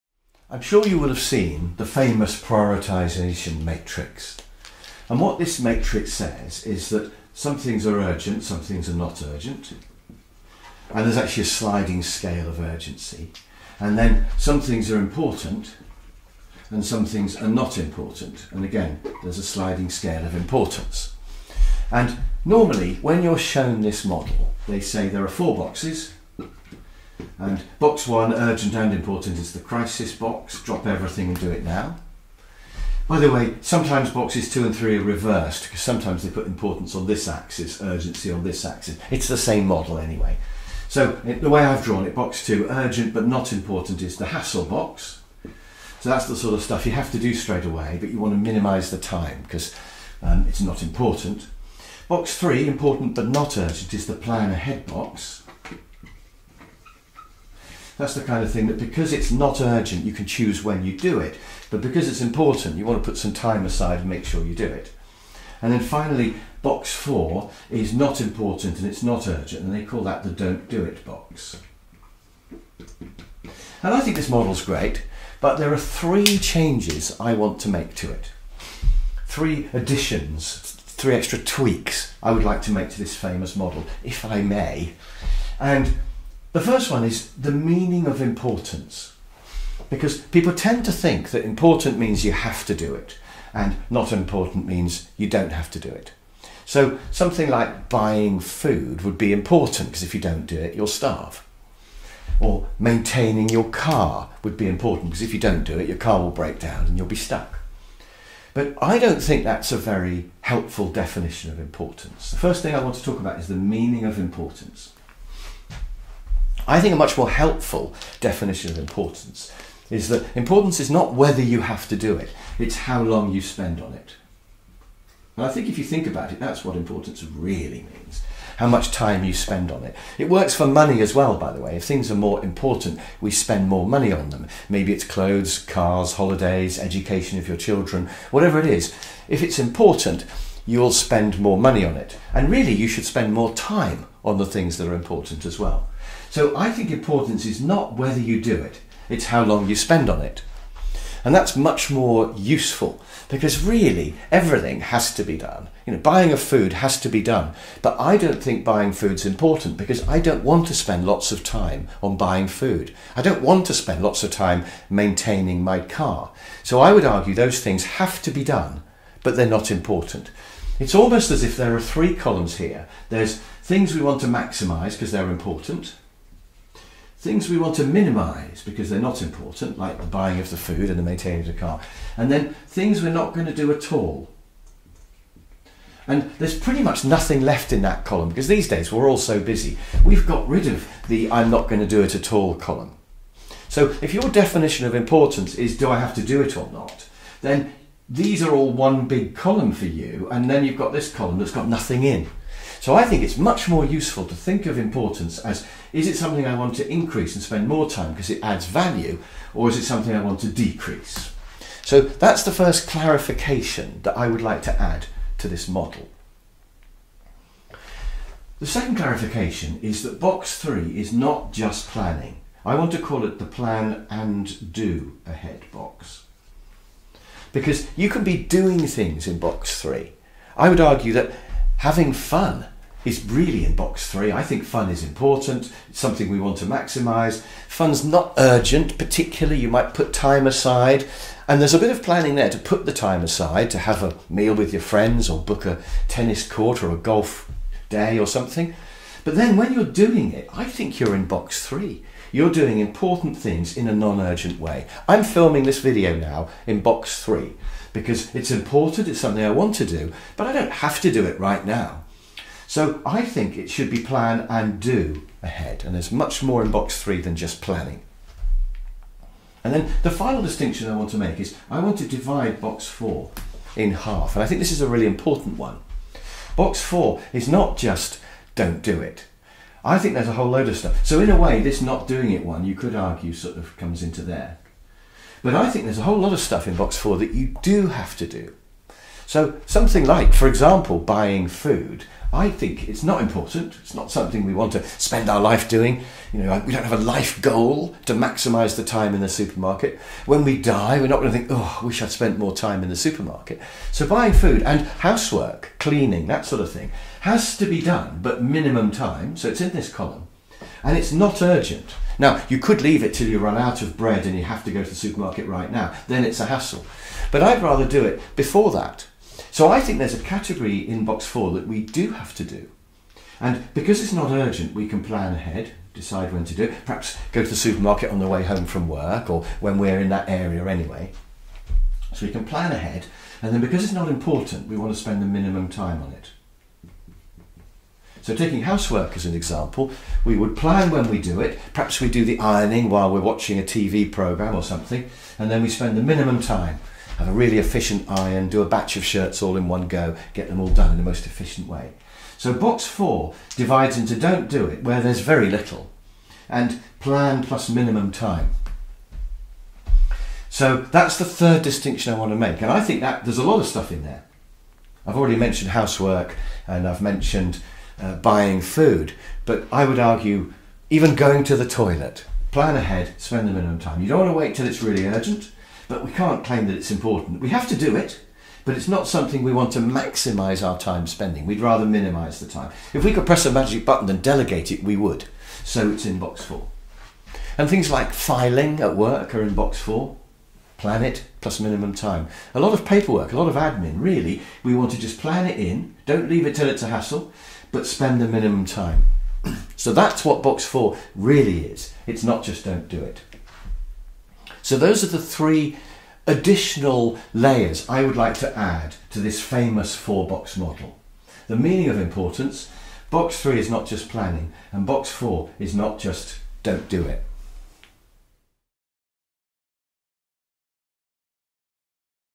Important vs Urgent - three changes / additions / improvements?
It's a classic model but I don't think it's the full story.  I've added three more thoughts which I hope you like.  What does 'important' really mean?  Is there more to box 3 than planning?  and is box 4 REALLY just "don't do it" - I actually think there is quite a lot of useful stuff box 4!

PS Want to be a freelance management trainer like me?